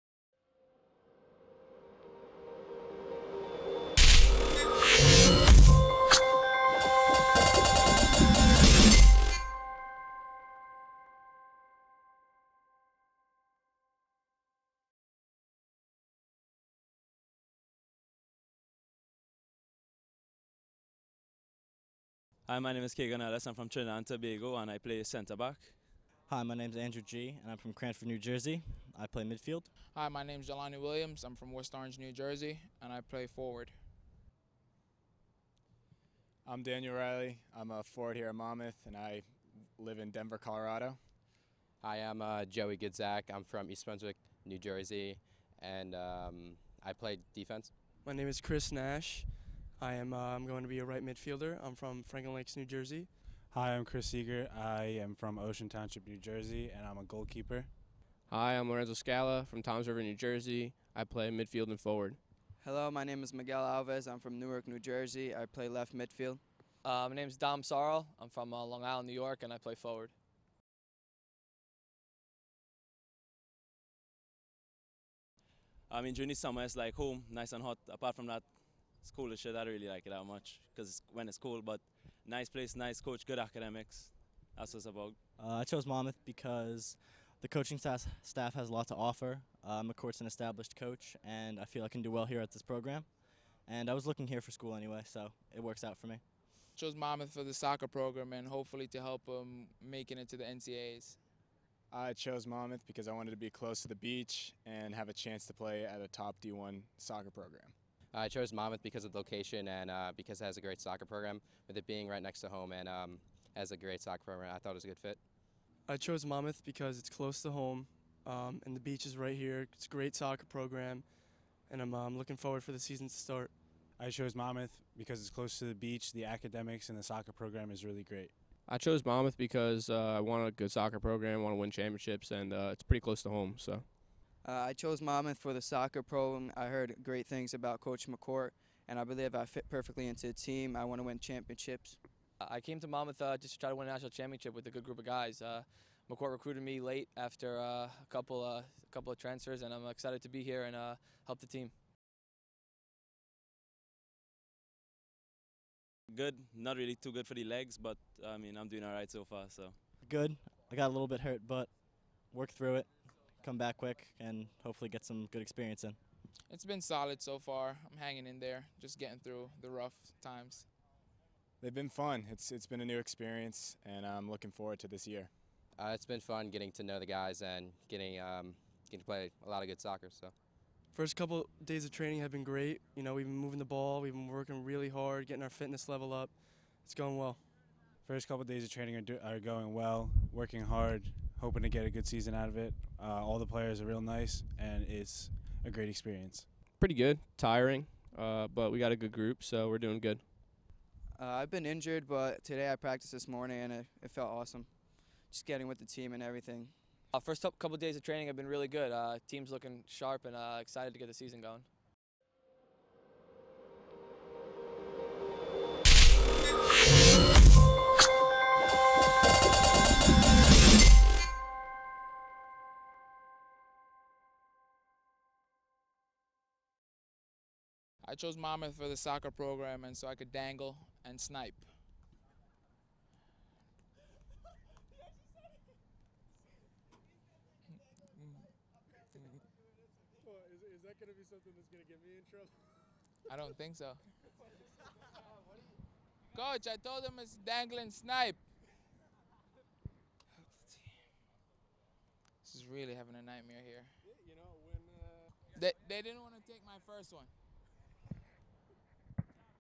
Meet The 2014 Monmouth Men's Soccer Newcomers
The newest additions to the Monmouth Men's Soccer family introduce themselves.